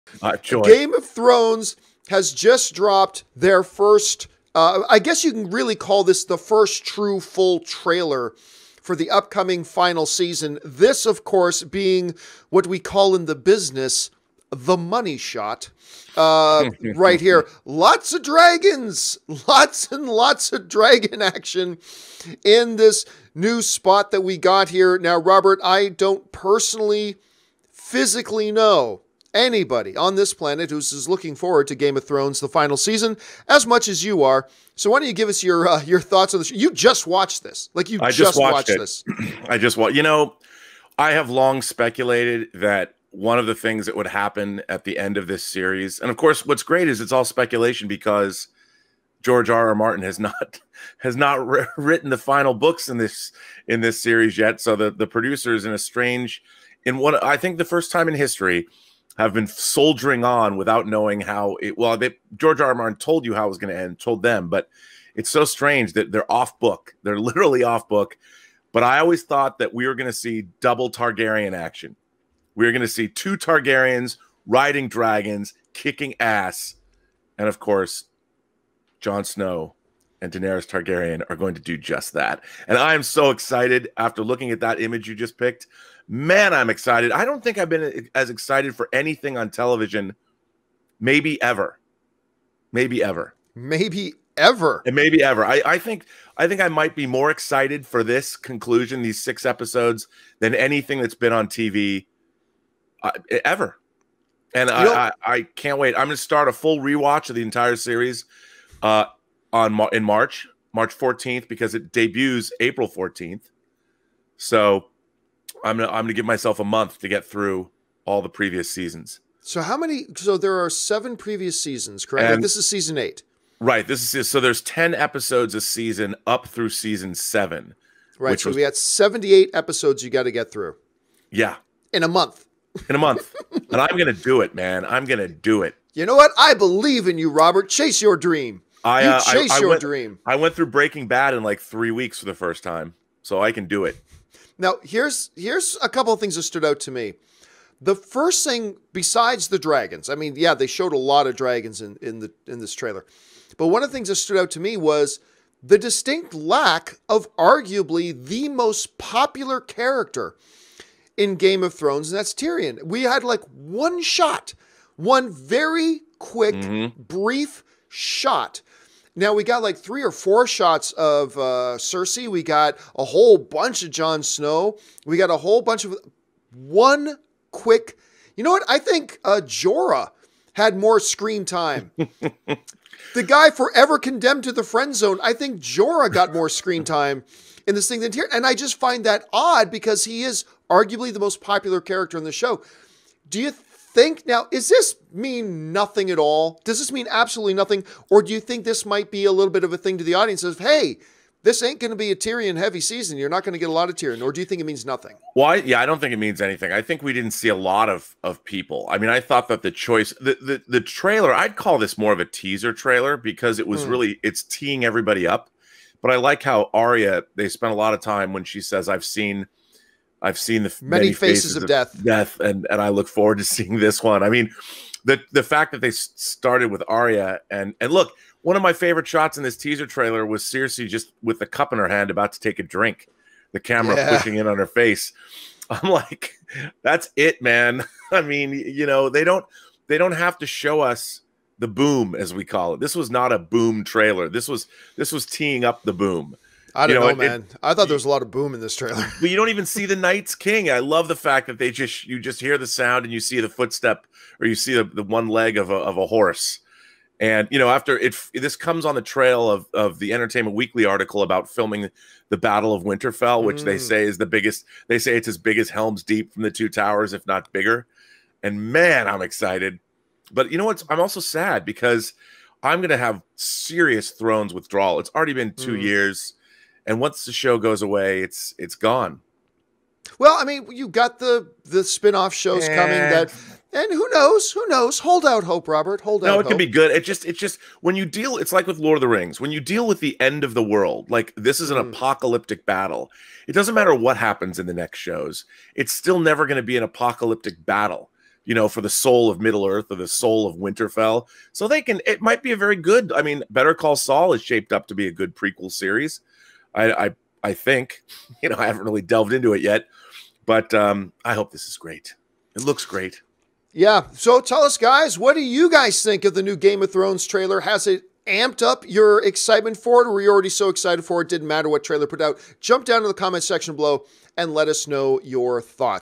Game Of Thrones Final Season Trailer Review
The epic fantasy series Game of Thrones will return for its six-episode, eighth and final season April 14, 2019. David Benioff & D.B. Weiss, David Nutter and Miguel Sapochnik will be the directors for the new season. Writers for the new season are David Benioff & D.B. Weiss, Bryan Cogman and Dave Hill.